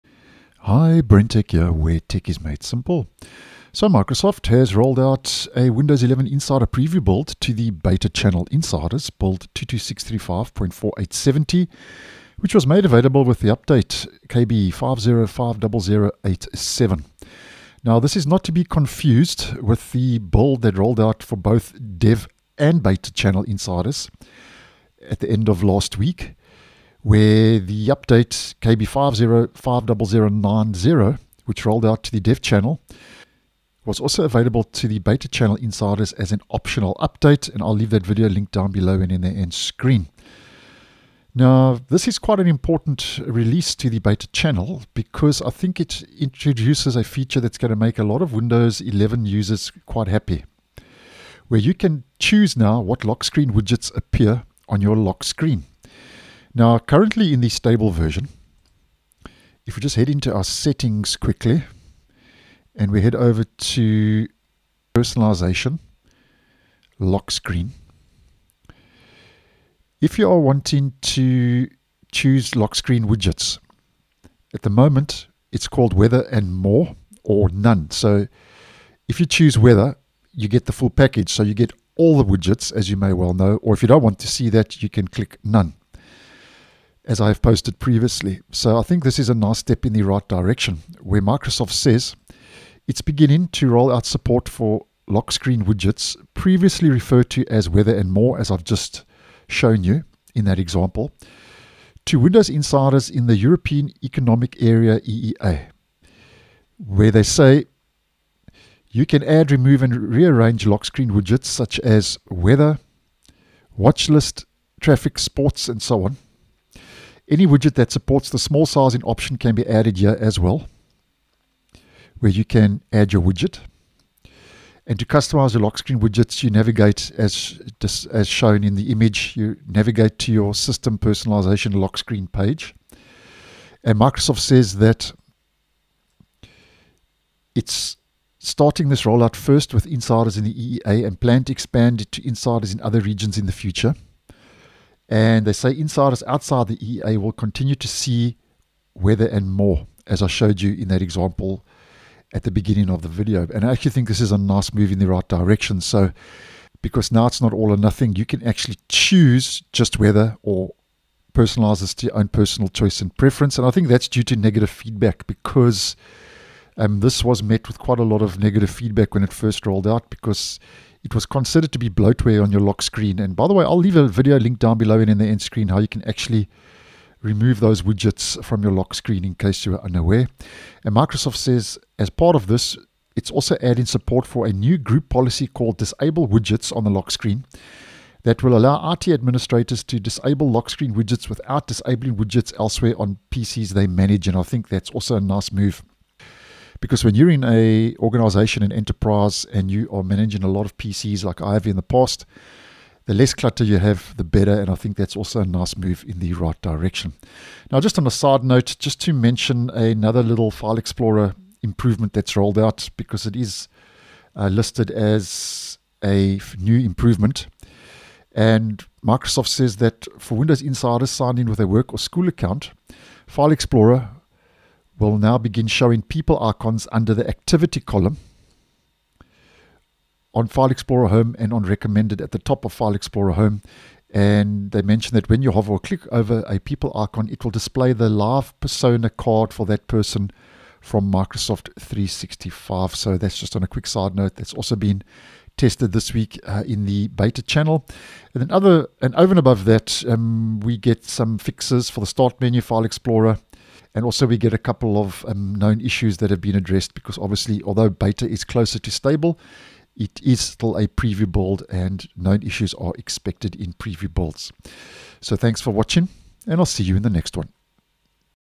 Windows 11 Build 22635.4870 Finally Lets You Customize The Lock Screen Widgets
Here's what's new in the latest Beta channel insider preview build with update KB5050087.